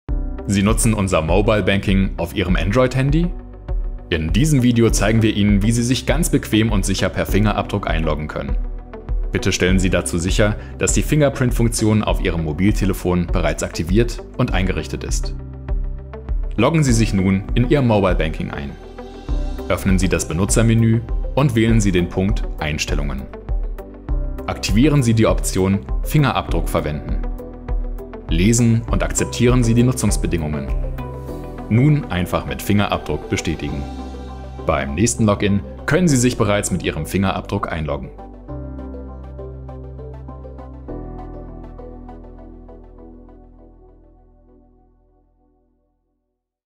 Login per Touch ID beim LLB Mobile Banking
Ein paarmal Tippen, schon ist ein Login mit Touch ID möglich.

Noch kein LLB Mobile Banking?
Jetzt herunterladen!